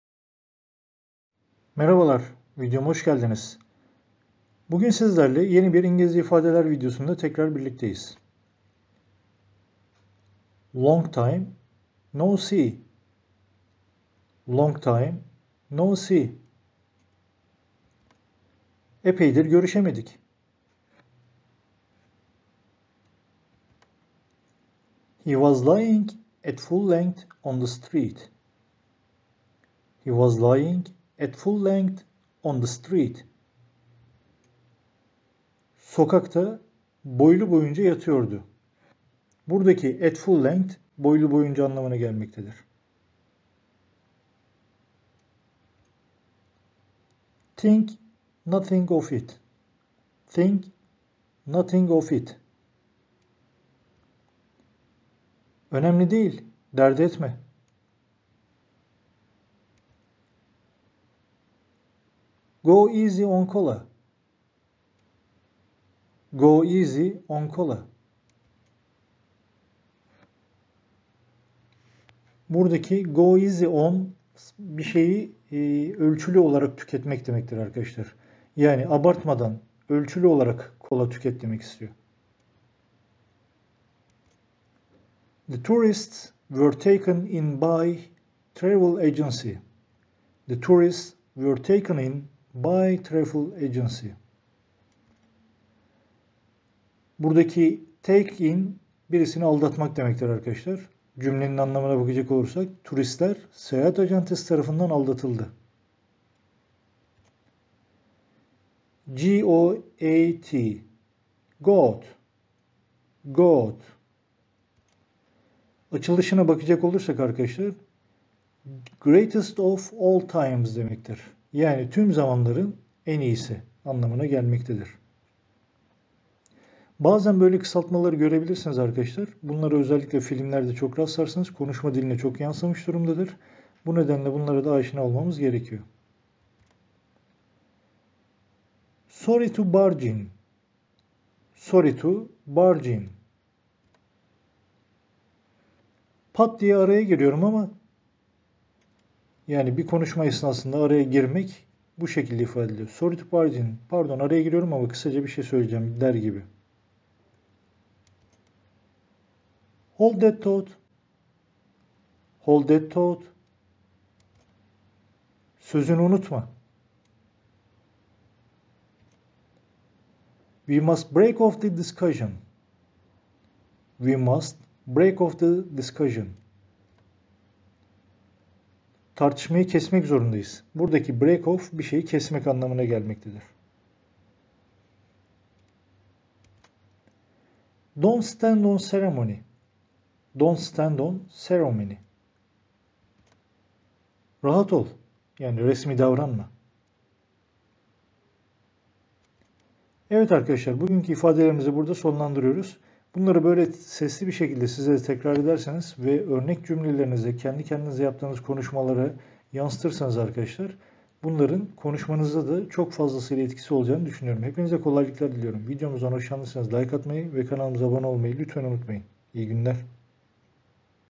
HERGÜN SADECE 3 dk ile İNGİLİZCE KONUŞUN! İngilizce Yaygın ve Günlük İfadeler - 7
Konuşma becerinizi geliştirmek için İngilizce en yaygın ve günlük ifadeleri öğreneceğiz. Filmlerde, makalelerde, kitaplarda sıkça rastlanılan ve konuşmanıza renk katacağını düşündüğüm ifadeleri derlemeye çalıştım.  Umarım beğenirsiniz. İyi çalışmalar.

kelimekelime öğrenme ingilizce, yökdil sosyal kelime ezberleme, yökdil sağlık kelimeler, yökdil fen kelimeleri